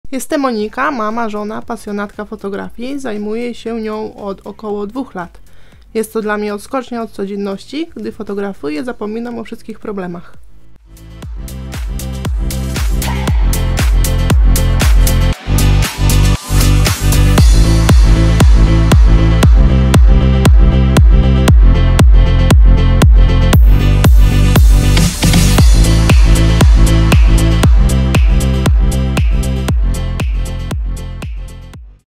Monika Peplińska - Mobbing Rowieśniczy " w szkole mnie wyśmiewali " Premiera 24.02.2024
MOBBING ROWIEŚNICZY " W SZKOLE MNIE WYŚMIEWALI " "DZISIAJ JESTEM PASJONATKĄ FOTOGRAFII , MAMA I ŻONĄ - NIE PODDAŁAM SIĘ ❤️" Monika Peplińska 🫶